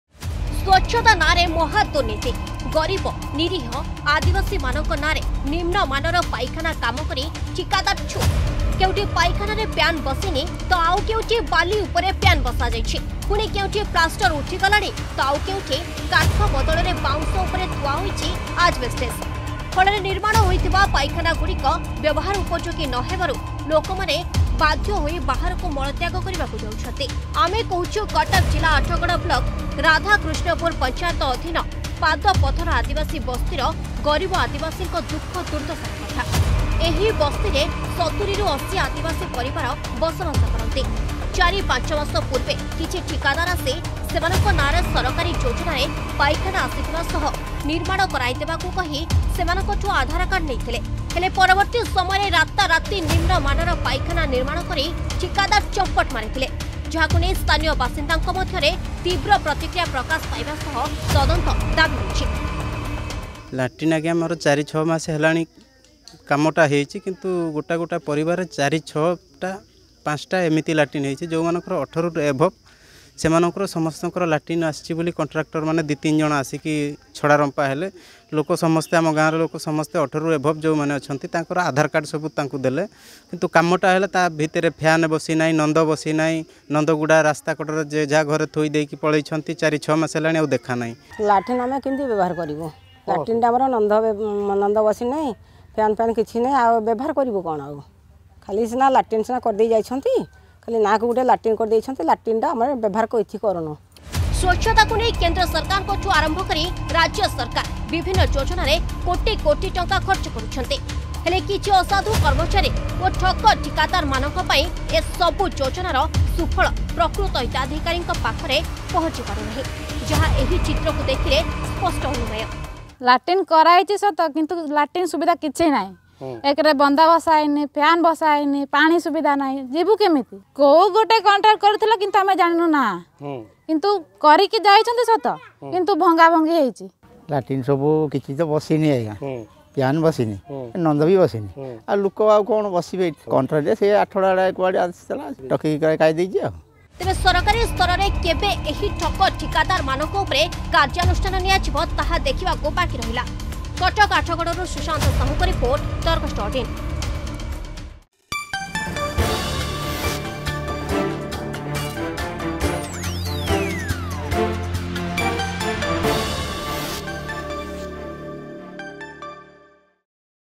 ସ୍ବଛତା ନାଁରେ ମହାଦୁର୍ନୀତି....
ସ୍ବଛତା ନାଁରେ ମହାଦୁର୍ନୀତି । ଗରିବ ନୀରିହ ଆଦିବାସୀ ମାନଙ୍କ ନାଁରେ ନିମ୍ନମାନର ପାଇଖାନା କାମ କରି ଠିକାଦାର ଛୁ । କେଉଁଠି ପାଇଖାନାରେ ପ୍ୟାନ ବସିନି ତ ଆଉ କେଉଁ ବାଲି ଉପରେ ପ୍ୟାନ ବସାଯାଇଛି । ପୁଣି କେଉଁଠି ପ୍ଲାଷ୍ଟର ଉଠି ଗଲାଣି ତ ଆଉଁ କେଉଁଠି କାଠ ବଦଳରେ ବାଉଁଶ ଉପରେ ଥୁଆ ହୋଇଛି ଆଜବେଷ୍ଟସ.

Category
News and Politics
Category
News and Politics
Licence